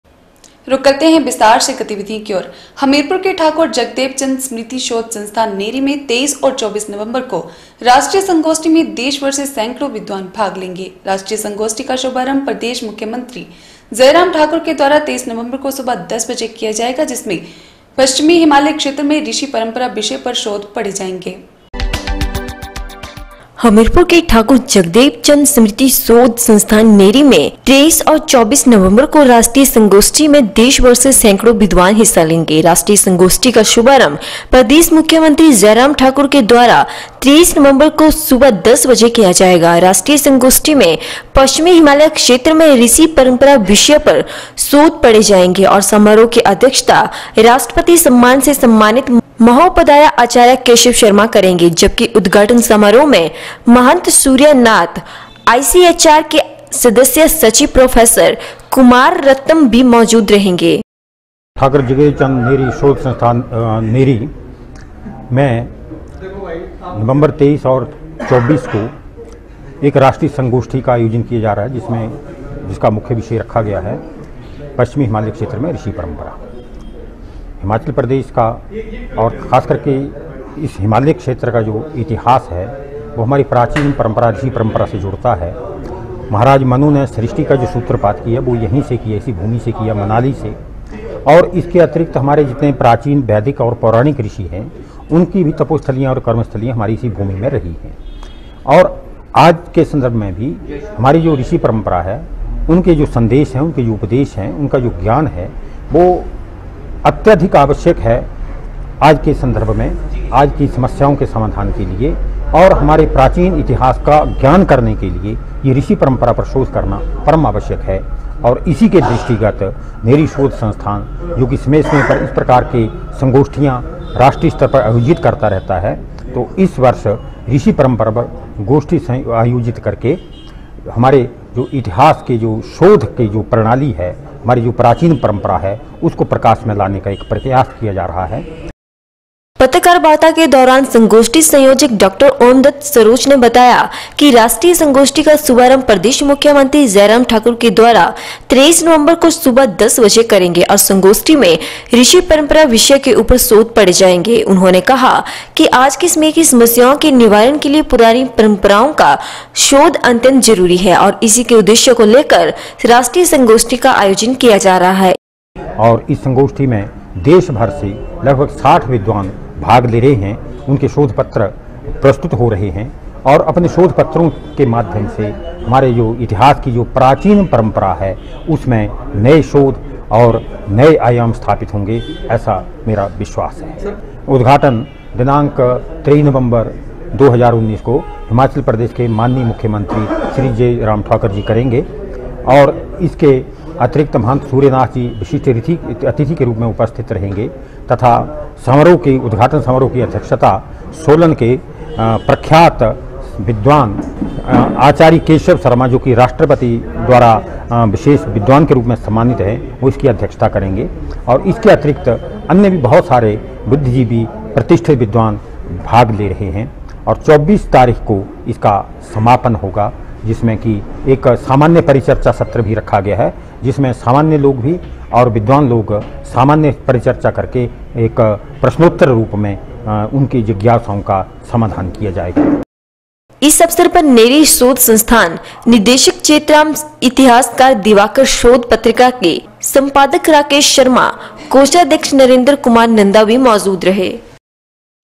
19 NOV N 1 23 और 24 नवंबर को राष्ट्रीय संगोष्ठी में देश भर से सैकडों विद्वान भाग लेंगे
हमीरपुर के ठाकुर जगदेव चंद स्मृति शोध संस्थान नेरी में 23 और 24 नवंबर को राष्ट्रीय संगोष्ठी में देश भर से सैकडों विद्वान भाग लेंगे। राष्ट्रीय संगोष्ठी का शुभारंभ प्रदेश मुख्यमंत्री जयराम ठाकुर के द्वारा 23 नवंबर को सुबह दस बजे किया जाएगा।